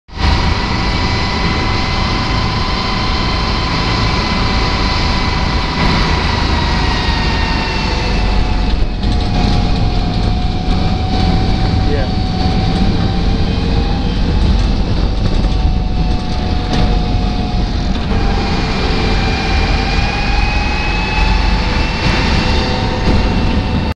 Armor on the Move
M1A1 Abrams Tank - convoy to the range. Ft. Knox, KY - 2006.
Filming credit - SGT Grenier, D. E-5 type - OD Green 1ea.